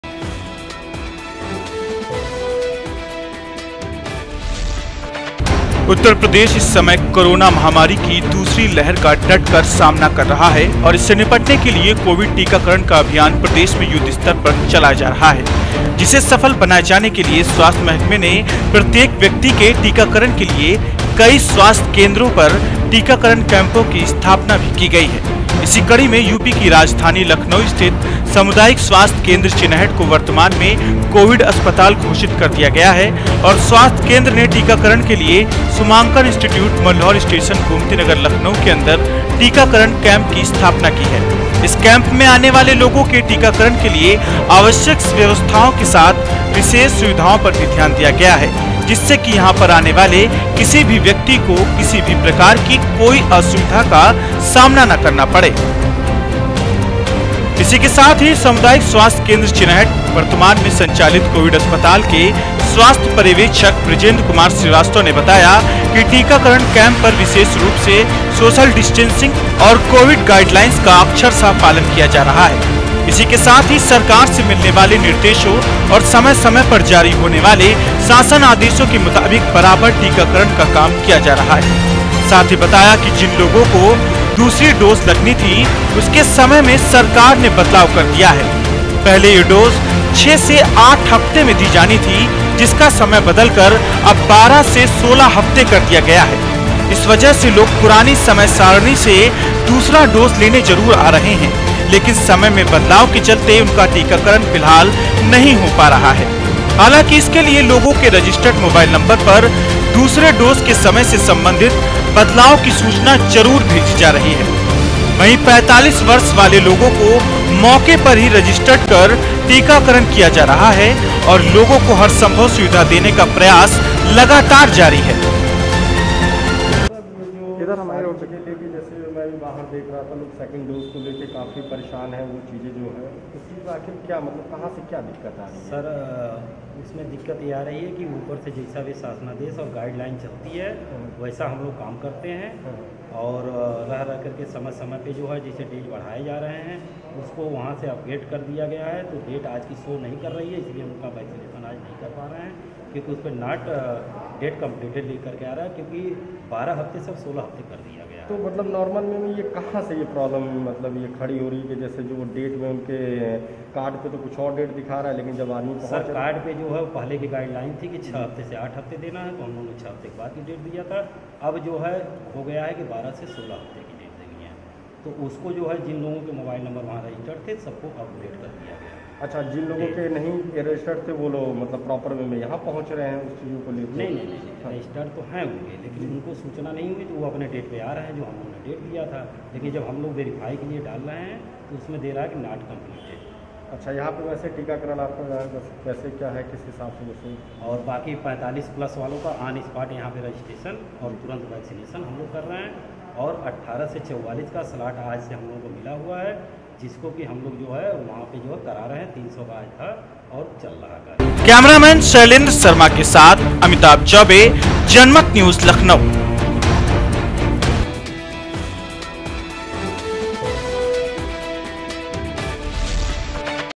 महामारी से निपटने के लिए सरकार ने “टीकाकरण” अभियान को दी “रफ़्तार”…
उत्तर प्रदेश इस समय कोरोना महामारी की दूसरी लहर का डटकर सामना कर रहा है और इससे निपटने के लिए कोविड टीकाकारण का अभियान प्रदेश में  युद्ध स्तर पर चलाया जा रहा है,  जिसे सफल बनाये जाने के लिए स्वास्थ्य महकमें ने  प्रत्येक व्यक्ति के टीकाकरण के लिए  कई स्वास्थ्य केंद्र पर *टीकाकरण कैम्पों* कि स्थापना भी की  है. इसी कड़ी में यूपी कि राजधानी लखनऊ  स्थित  सामुदायिक स्वास्थ्य केंद्र चिनहट को वर्तमान में कोविड अस्पताल घोषित कर दिया गया है और स्वास्थ्य केंद्र ने टीकाकरण के लिए  शुमाकर इंस्टिट्यूट, मल्हौर स्टेशन , गोमती नगर, लखनऊ के अन्दर  *टीकाकरण कैम्प* की स्थापना की है, इस कैम्प में  आने वाले लोगो के टीकाकरण के लिए आवश्यक व्यवस्थाओं के साथ विशेष सुविधाओं पर भी ध्यान दिया गया है. जिससे कि यहाँ पर आने वाले किसी भी  व्यक्ति को किसी भी प्रकार कि कोई असुविधा का सामना न करना पड़े.